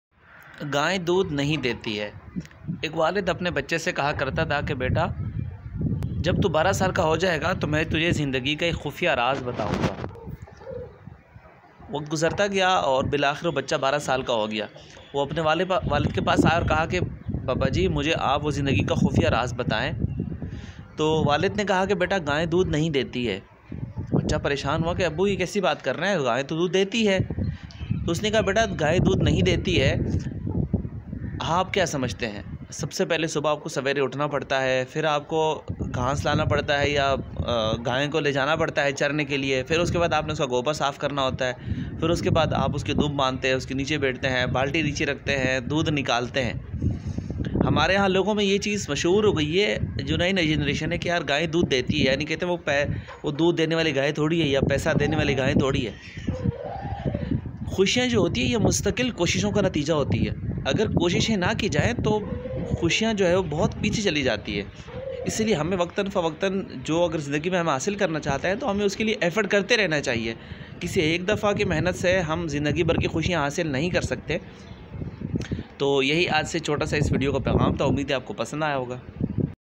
Happiness is the consequences of continues efforts | SHORT STORY | MOTIVATIONAL SHORT STORY | SHANAS
this is short motivational story about efforts and happiness in life